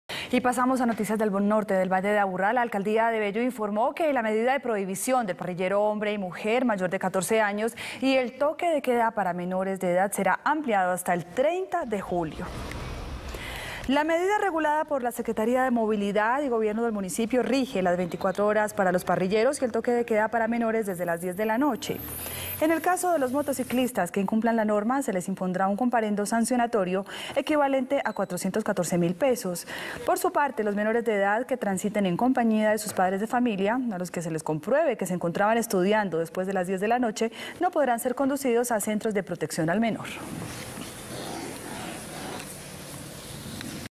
En Bello amplían medidas de seguridad hasta el 30 de julio [Noticias] - Telemedellín
La Alcaldía de Bello informó que la medida de prohibición del parrillero hombre y mujer mayor de 14 años y el toque de queda para menores de edad será ampliada hasta el 30 de julio.